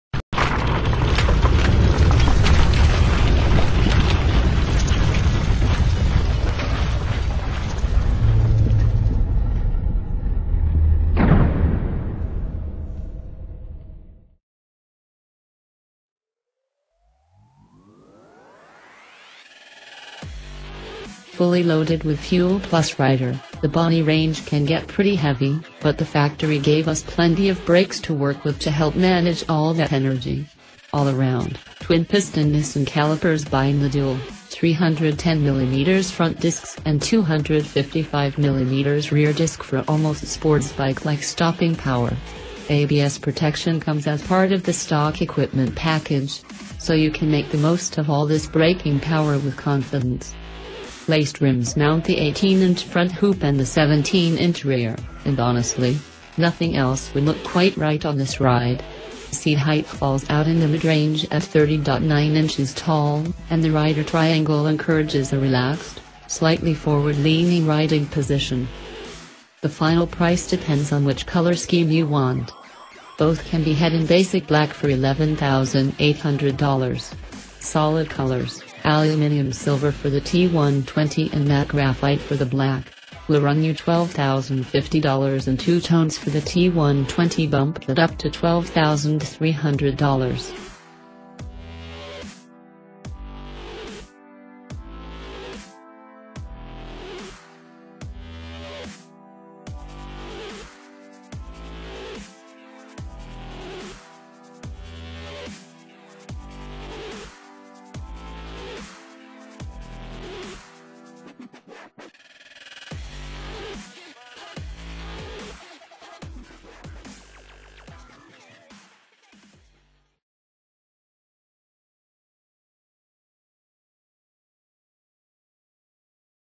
Otto Bike l 2019 Triumph Bonneville T120 T120 Black Design Overview
Otto Bike l 2019 Triumph Bonneville T120  T120 Black Design Overview-Laced rims mount the 18-inch front hoop and the 17-inch rear, and honestly, nothing else would look quite right on this ride. Seat height falls out in the midrange at 30.9-inches tall, and the rider triangle encourages a relaxed, slightly forward-leaning riding position.

██████ GET DAILY UPDATES! NOW! ██████